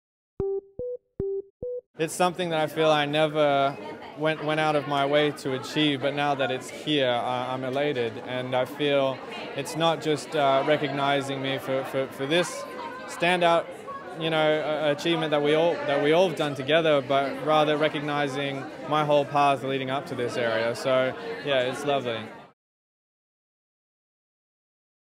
Kodi Smit-McPhee interview on The Power of the Dog nomination at BAFTAs 2022 party
This footage is part of the official BAFTAs EPK content for press usage.